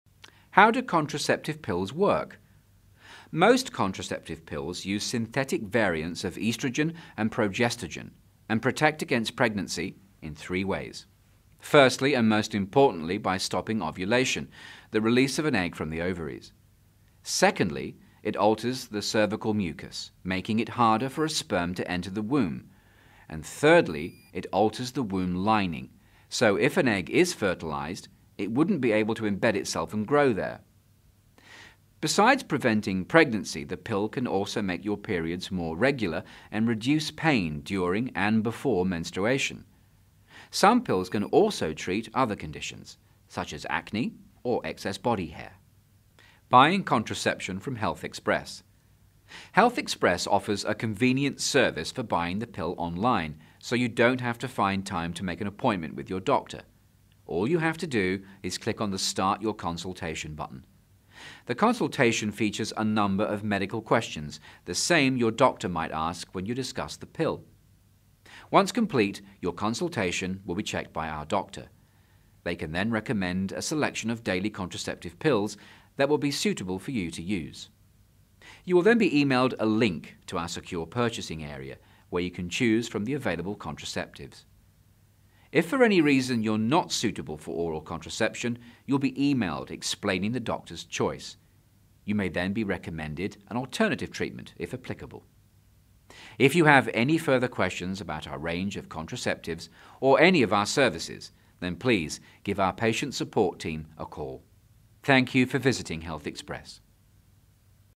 How Do Contraceptive Pills Work? - HealthExpress.co.uk
Information about how contraceptive pills work to prevent pregnancy. Find out what other benefits the pill can provide, such as reducing pain before and during menstruation.